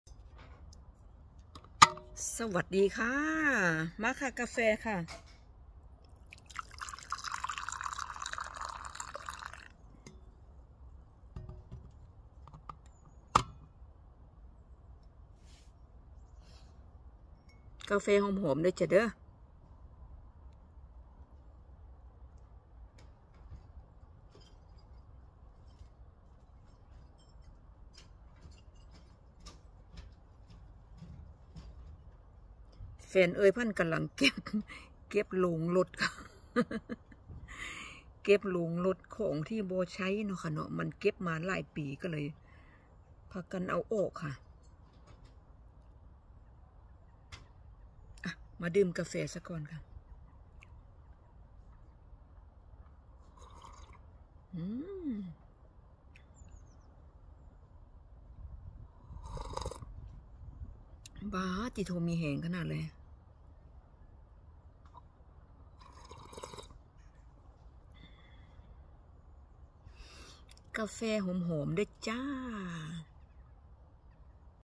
กาแฟในสวน
สวัสดีคะเพื่อนๆทุกคนคะมาคะดื่มกาแฟด้วยกันคะในสวนหลังบ้านจ้าอากาศดีมากเลยจ้าสบายๆจ้าโชคดีคะทุกคนคะ♥️😍